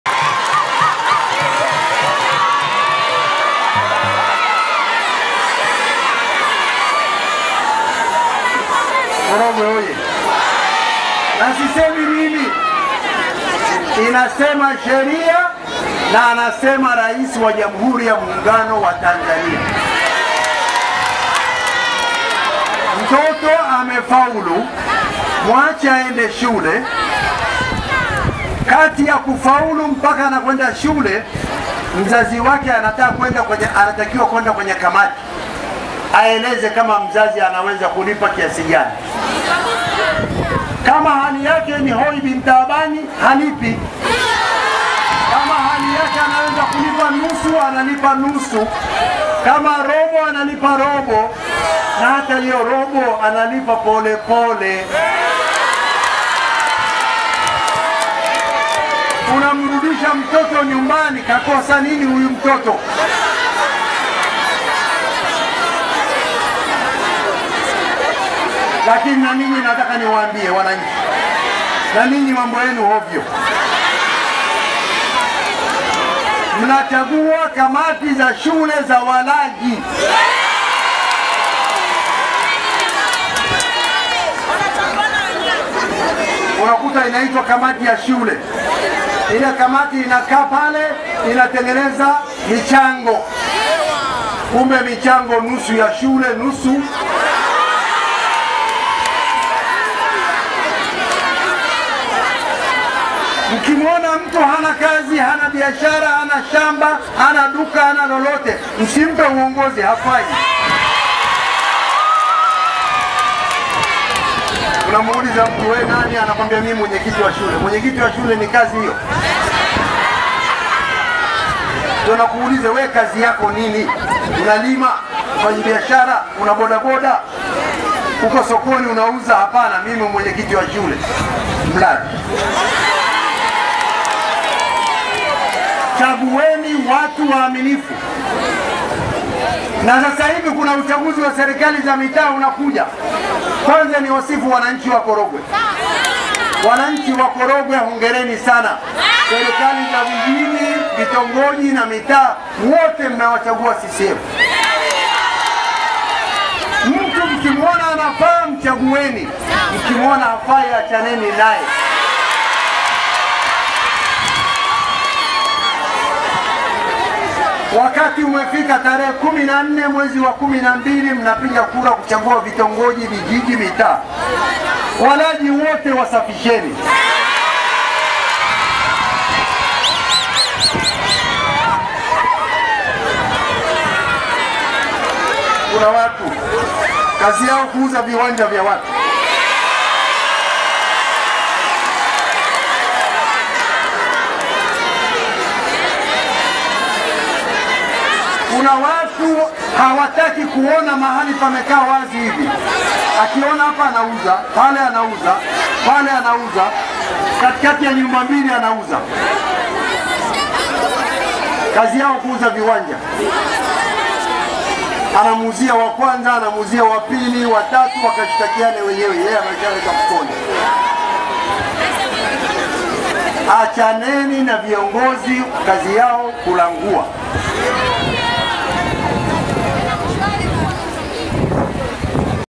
KINANA CHAGUENI VIONGOZI BORA
Kinana akihutubia wananchi wa Korogwe mjini na kutaka wananchi kuchagua viongozi bora.